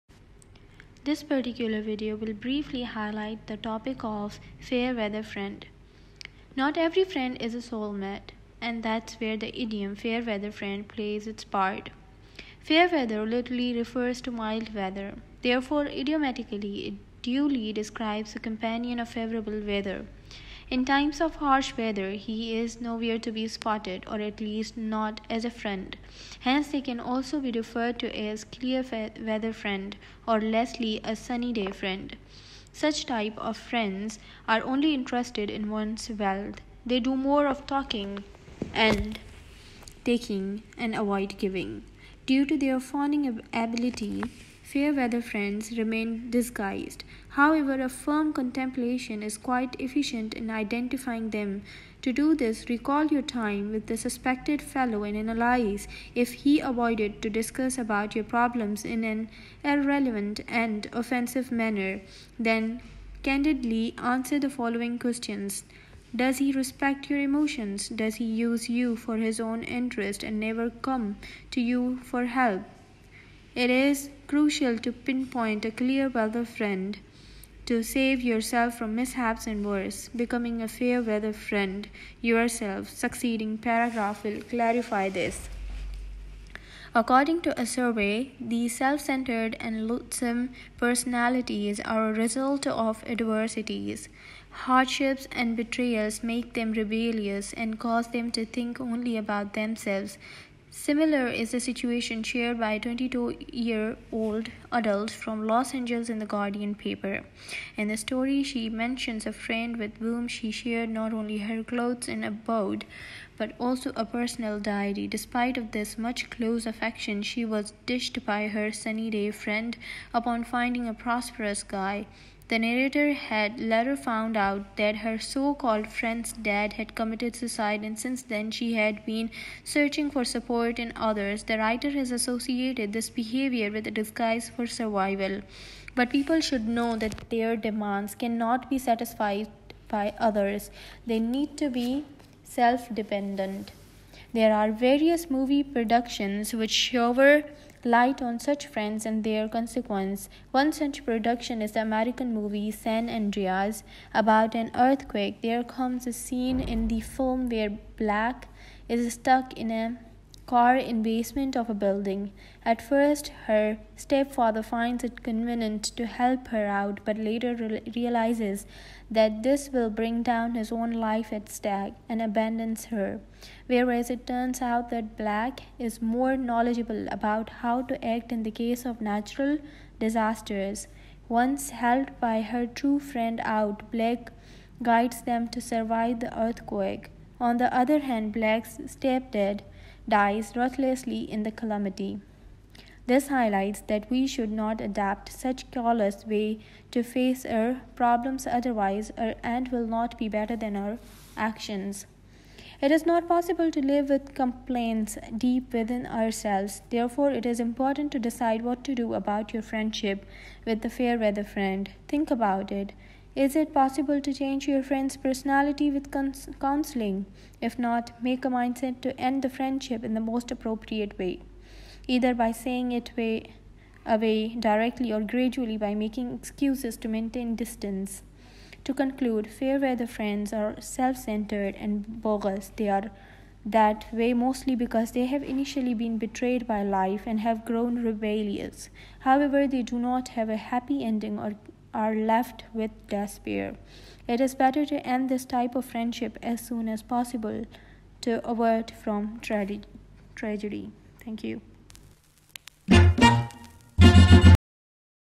Fair Weather Friend. A short essay.
The video is a short essay to boost ideas about the common idiomatic expression ‘Fair Weather Friend’.